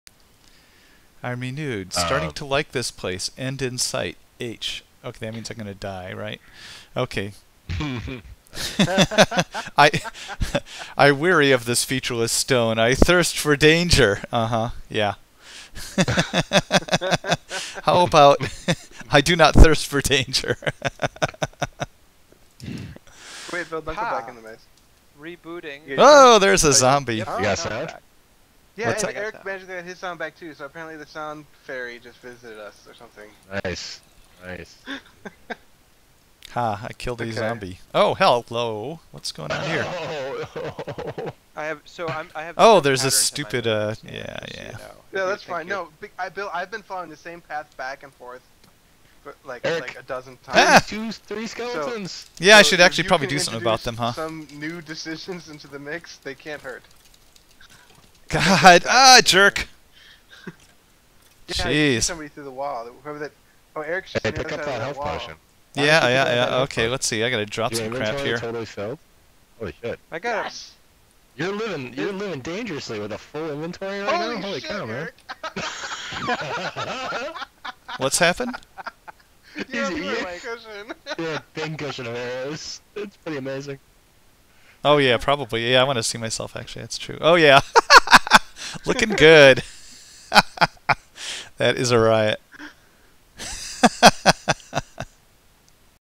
Pincushion
Fighting monsters with watermelon is less effective than you might imagine.

Fusty and Thex plan on releasing the maze as an adventure map sometime soon. Visit vokselia.com for more information.